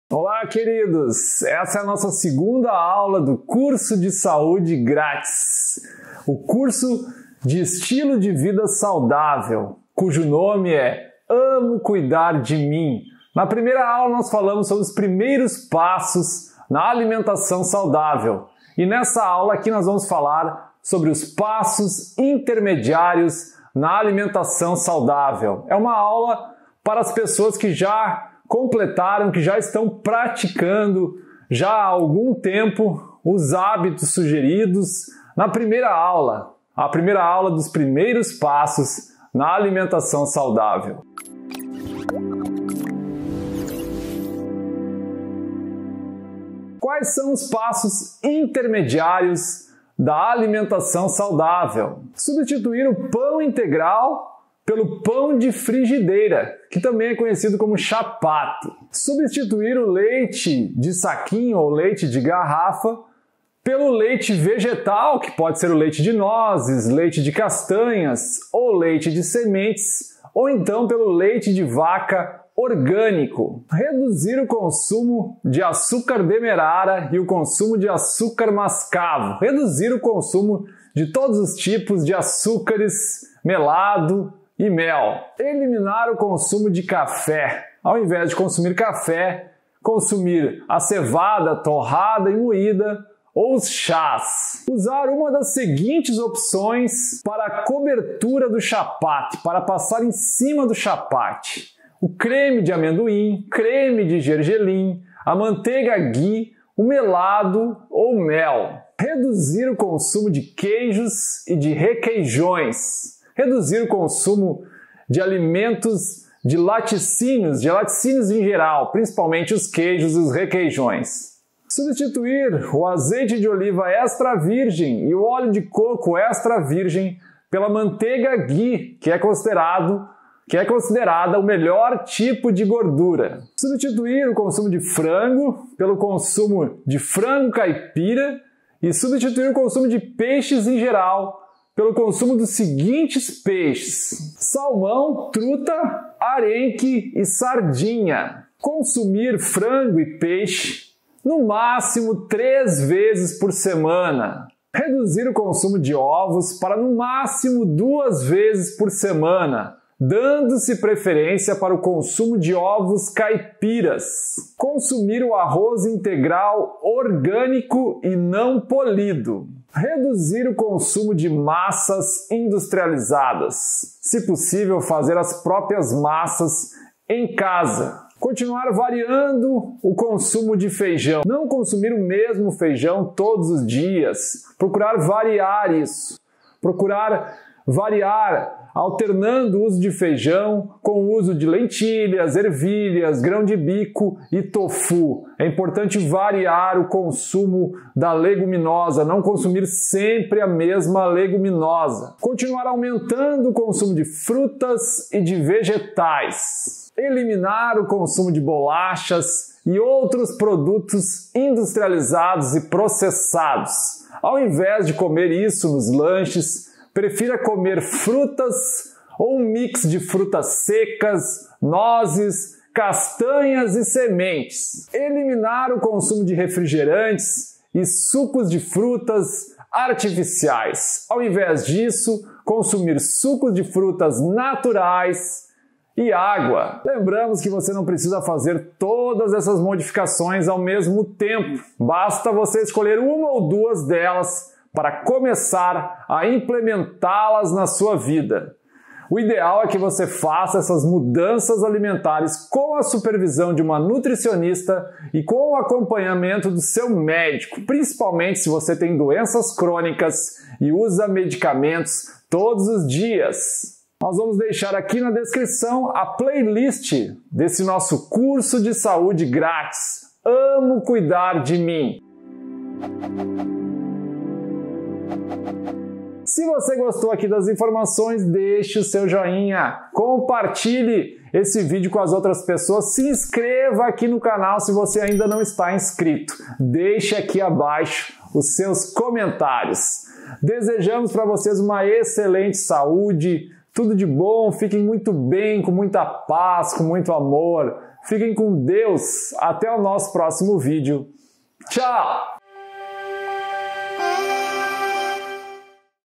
AULA 2 - CURSO DE SAÚDE | PASSOS INTERMEDIÁRIOS NA ALIMENTAÇÃO SAUDÁVEL
Nesse vídeo apresentamos os passos intermediários para uma alimentação saudável. Afinal, como disse o Pai da Medicina Hipócrates: "Faça do seu alimento o seu medicamento".